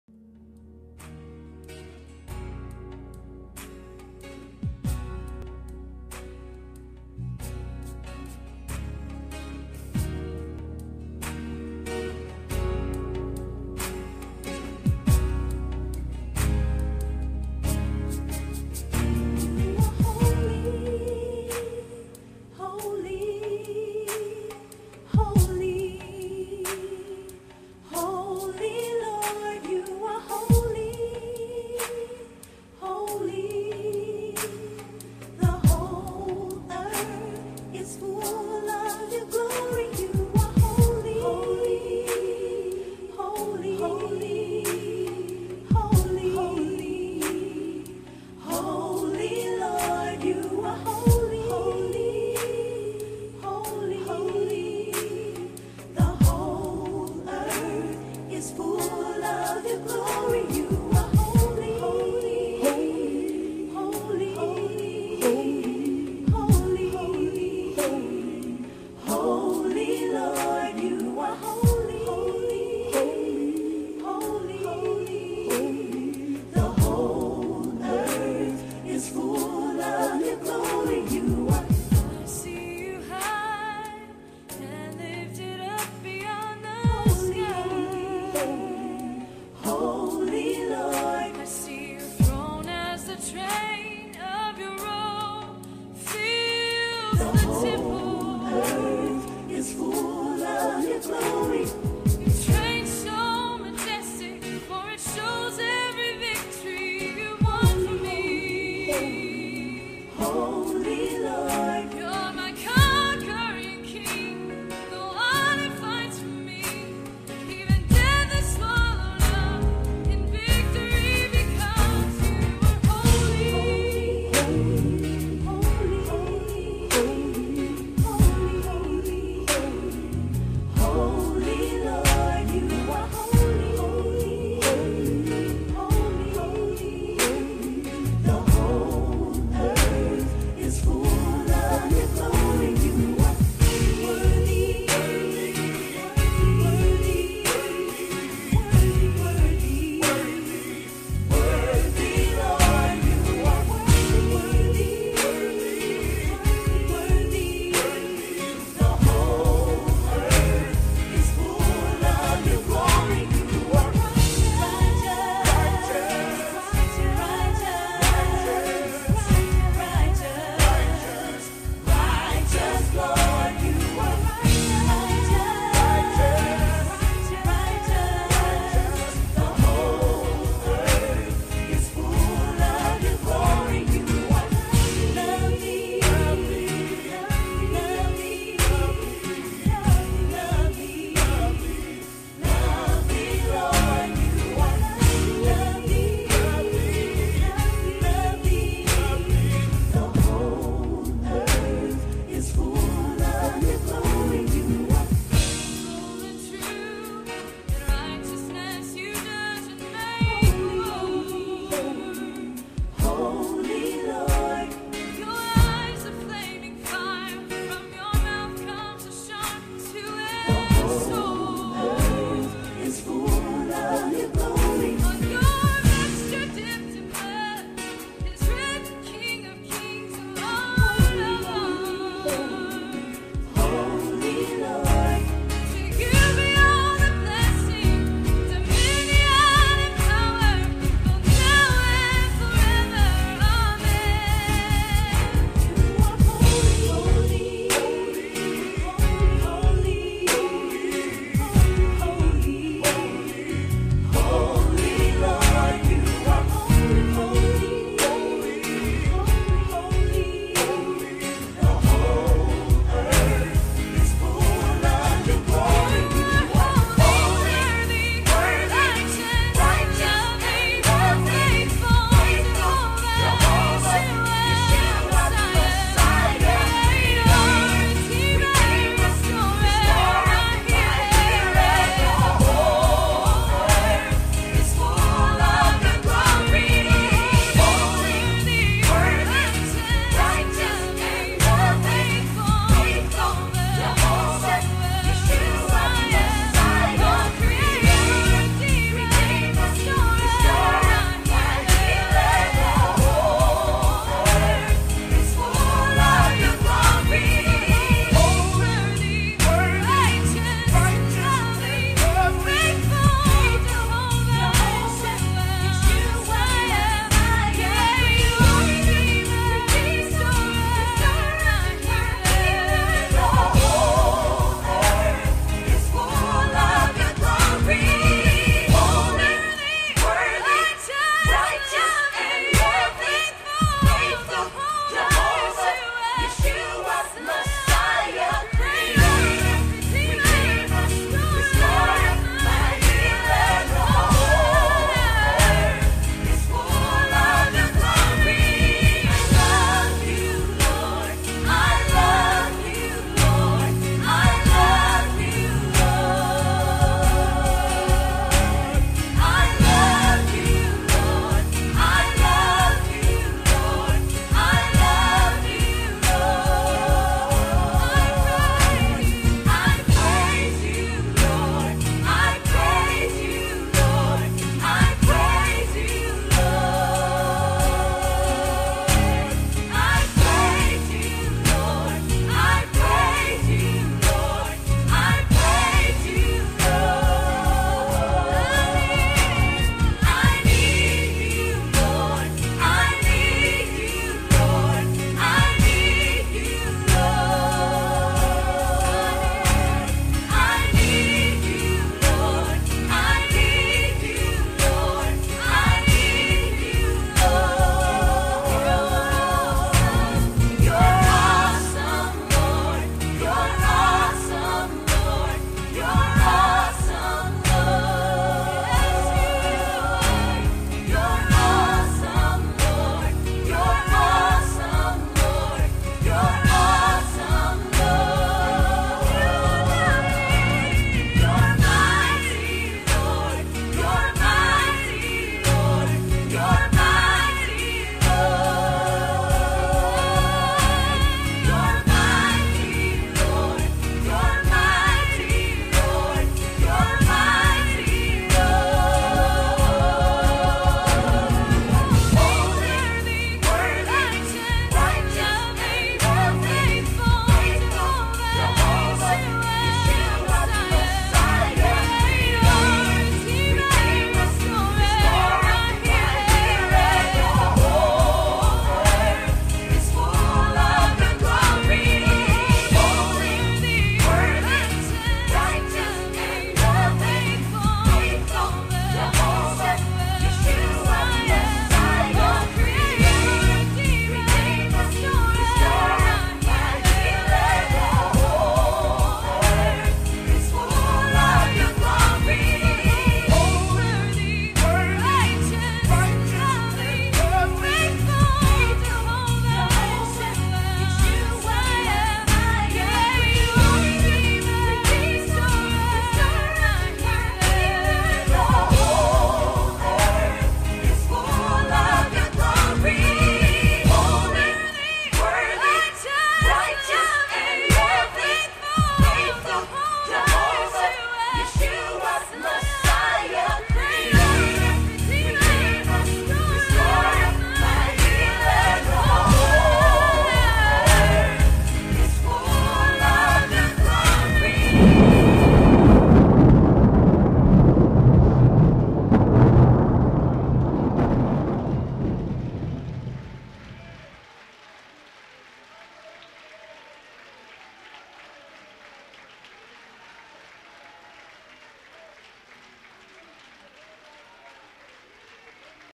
Ladies Drama
Drama born out of a vision the Lord gave to the Pastor's wife. God is HOLY, Worthy, Lovely.